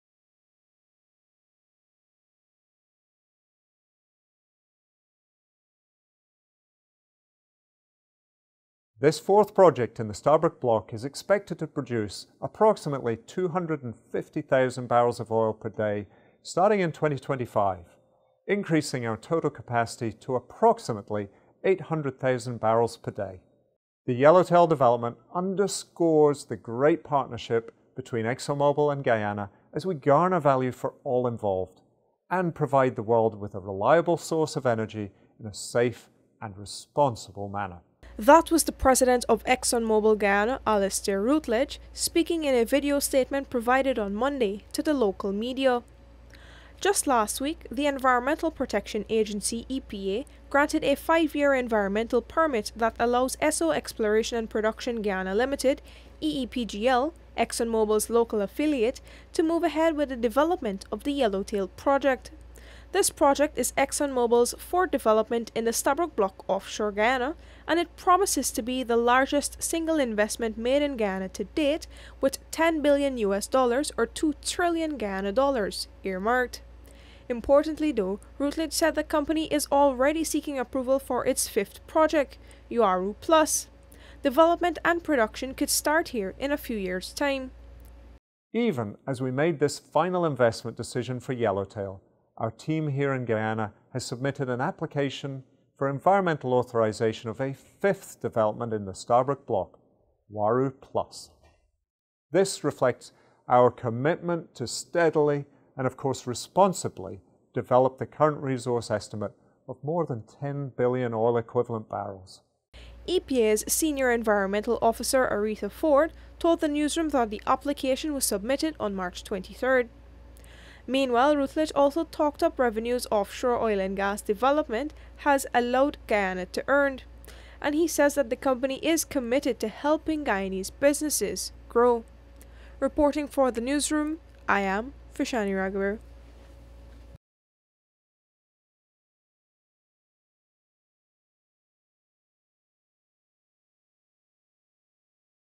EXXON EARMARKS US$10B TO DEVELOP YELLOWTAIL; NOW SEEKS APPROVAL FOR FIFTH PROJECT
ExxonMobil plans to spend US$10 billion (about GY$2 trillion) at its recently approved Yellowtail project, in the Stabroek Block offshore Guyana, but the company is also now seeking environmental approval for its fifth project: Uaru+.

This is according to President of ExxonMobil Guyana Alistair Routledge, in a video statement provided on Monday to the local media.